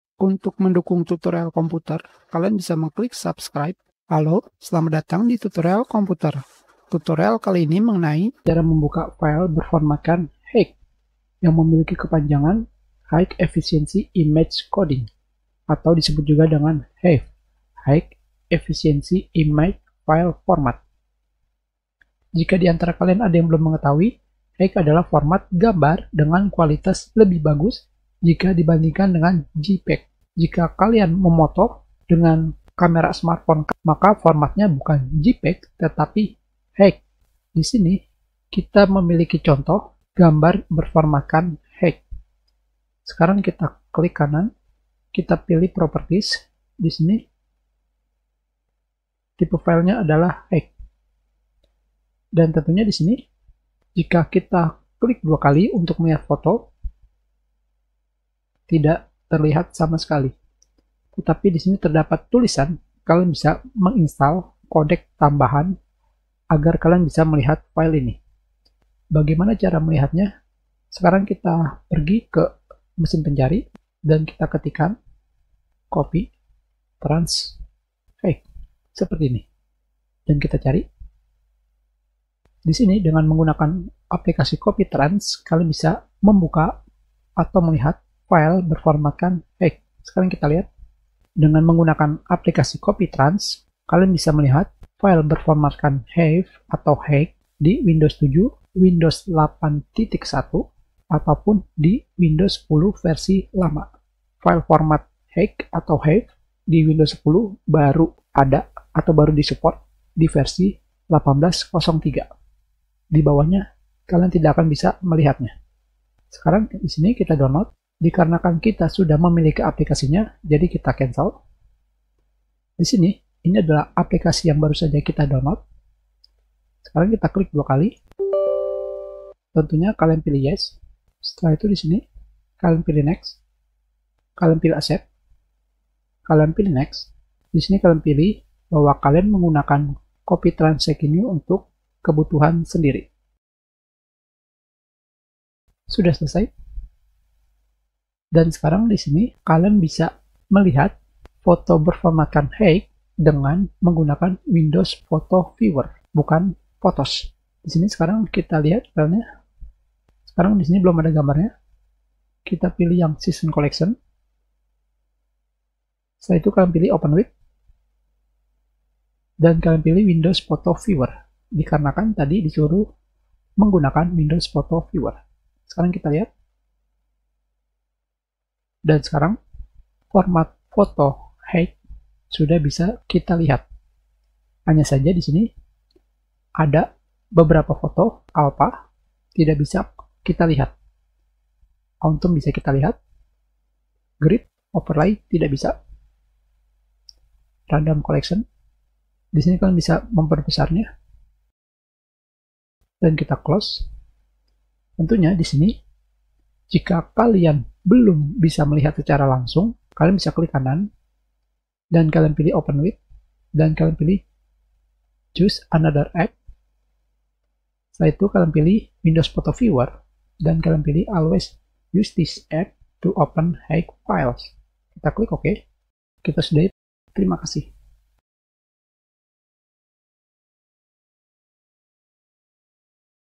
Membuka File Foto Dengan Format atau Ekstensi HEIC HEIF Di Windows 10, 8 1, dan 7
Tutorial komputer kali ini mengenai membuka file folo dengan format atau ekstensi HEIC/HEIF di Windows 10.